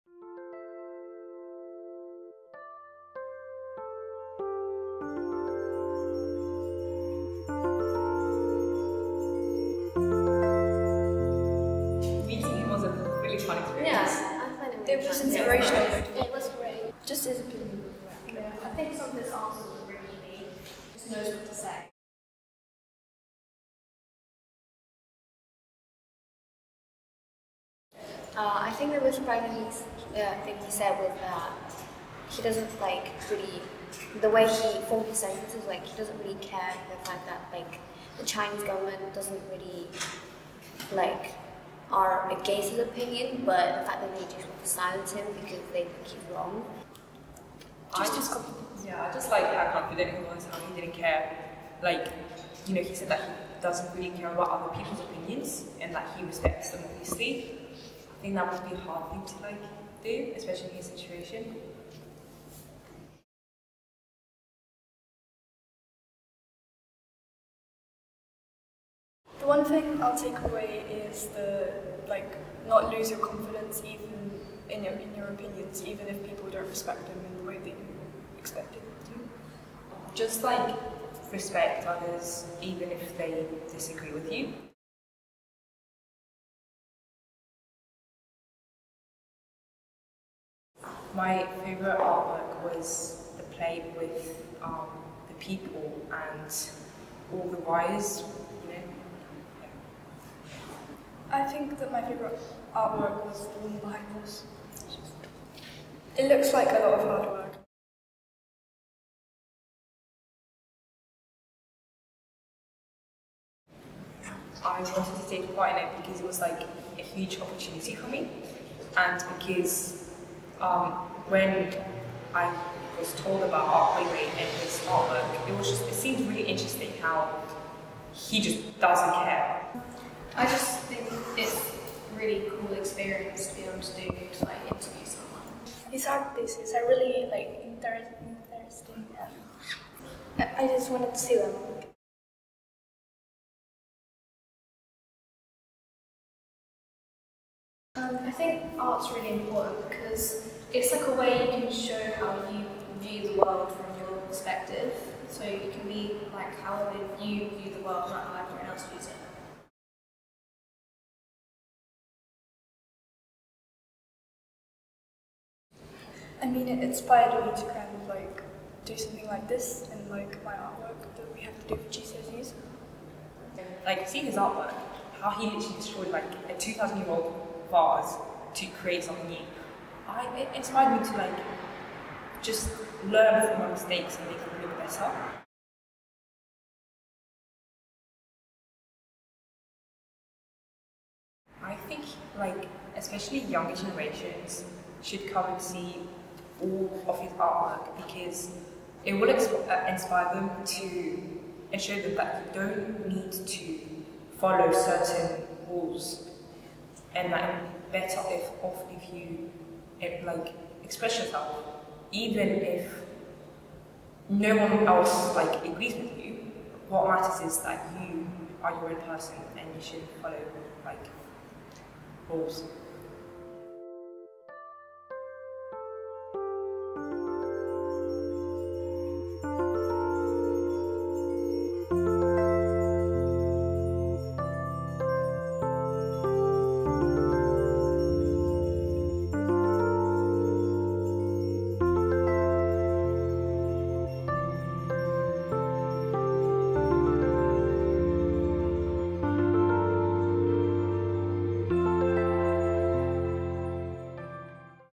Ai Weiwei at Kettle's Yard: Afterthoughts
Students from North Cambridge Academy reflect on their experience interviewing artist Ai Weiwei at Kettle's Yard.